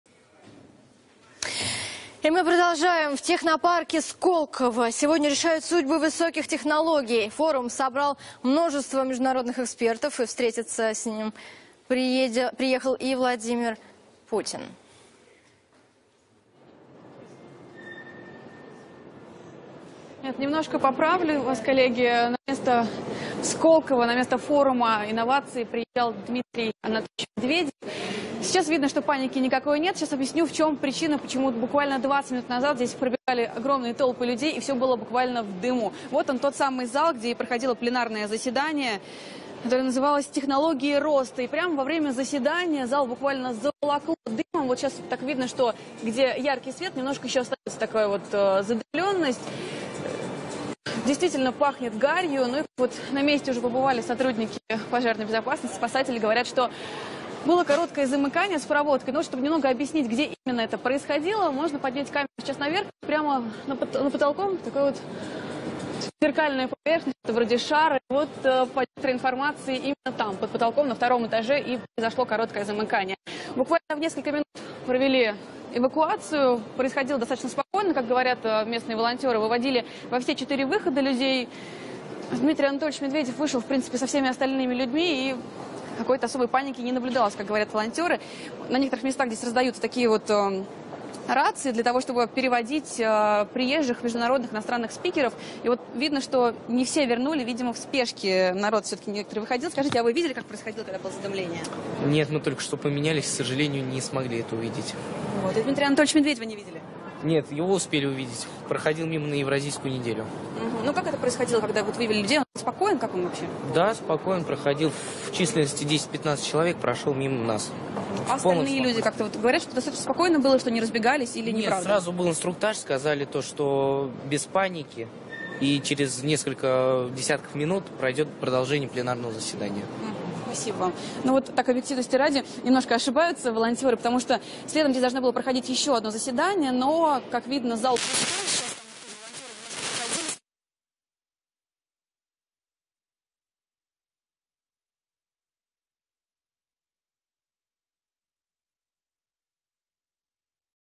Проходит форум “Открытые инновации 2016”
Форум собрал множество международных экспертов